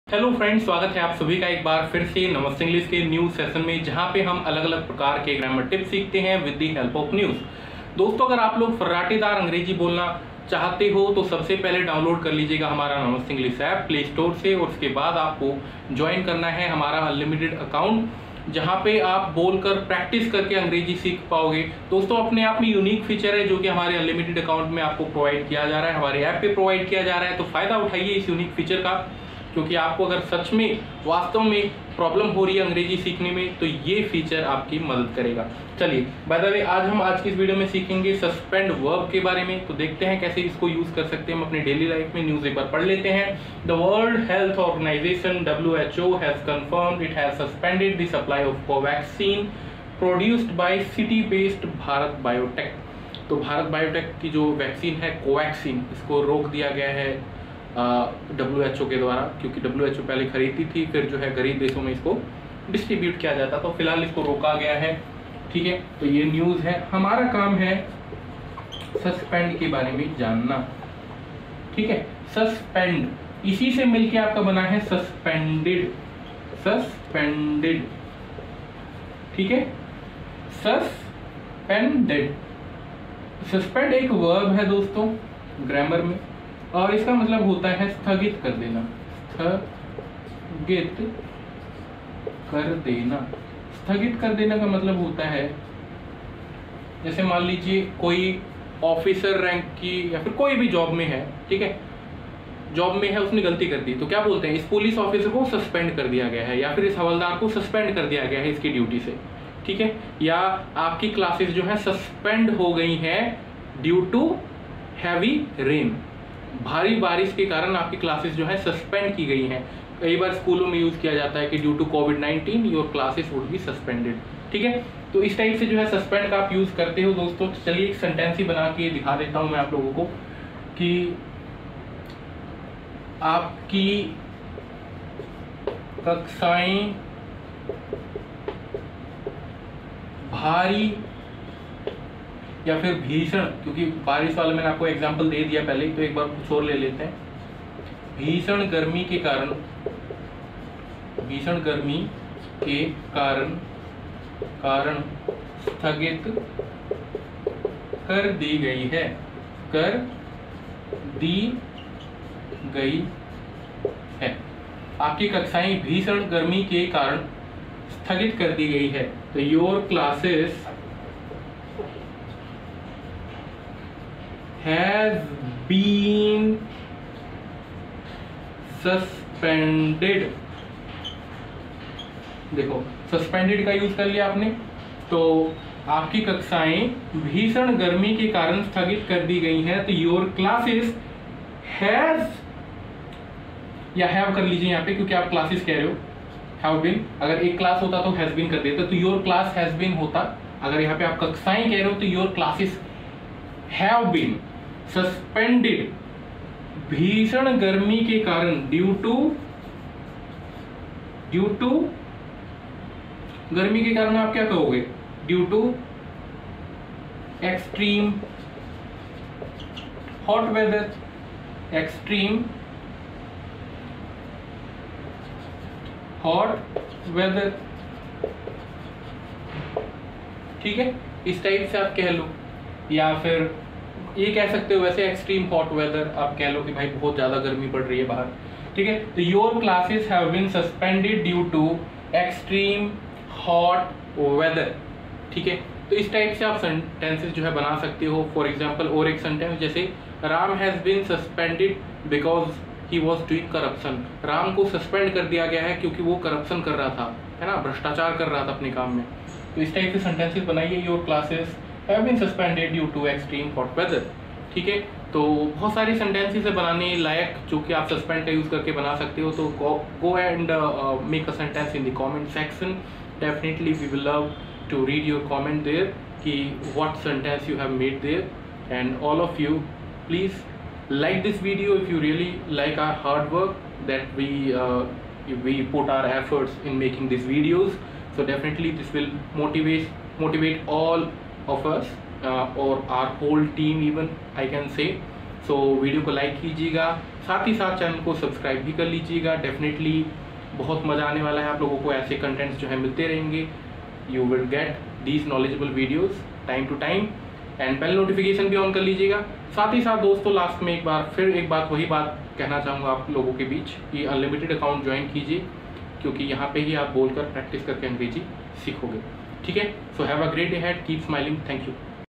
न्यूज से अंग्रेजी सीखों - WHO suspends Covaxin supply to UN agencies
Use of Suspend :
transitive verb
1: to debar temporarily especially from a privilege, office, or function
suspend a student from school
2a: to cause to stop temporarily
suspend bus service
b: to set aside or make temporarily inoperative
suspend the rules

NEWS in Details :
The World Health Organization (WHO) has confirmed it has suspended the supply of Covaxin, produced by city-based Bharat Biotech, through UN procurement agencies and recommended to countries that received the covid vaccine to take action as appropriate.A statement issued by WHO on Saturday said the suspension is in response to the outcomes of its post-EUL (emergency use authorisation) inspection held between March 14–22, 2022 and the need to conduct process and facility upgrade to address recently identified GMP (good manufacturing practice) deficiencies.
विश्व स्वास्थ्य संगठन (डब्ल्यूएचओ) ने पुष्टि की है कि उसने संयुक्त राष्ट्र की खरीद एजेंसियों के माध्यम से शहर स्थित भारत बायोटेक द्वारा उत्पादित कोवैक्सिन की आपूर्ति को निलंबित कर दिया है और उन देशों को सिफारिश की है जो उचित कार्रवाई करने के लिए कोविड वैक्सीन प्राप्त करते हैं। डब्ल्यूएचओ द्वारा जारी एक बयान शनिवार ने कहा कि निलंबन 14-22 मार्च, 2022 के बीच आयोजित ईयूएल (आपातकालीन उपयोग प्राधिकरण) निरीक्षण के परिणामों के जवाब में है और हाल ही में पहचाने गए जीएमपी (अच्छे विनिर्माण अभ्यास) की कमियों को दूर करने के लिए प्रक्रिया और सुविधा उन्नयन की आवश्यकता है।